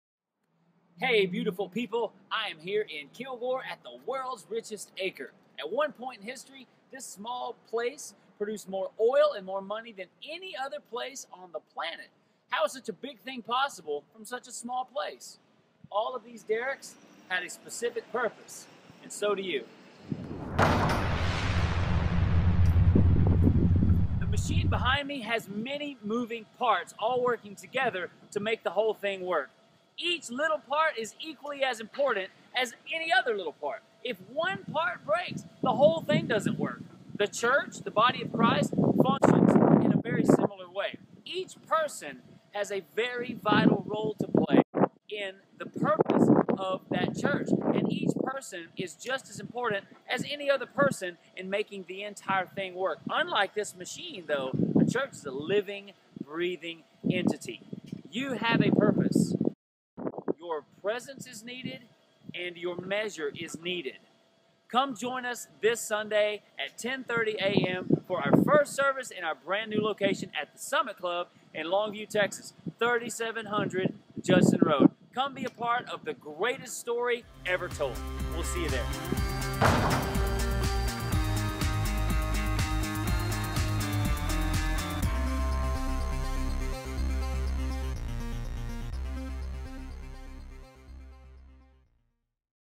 Revive Church YOU Have a Purpose!
No matter where you fit in, how big or how small you think it may be. YOU have a purpose, and God has a plan. Your measure is needed, and the whole can't work without you.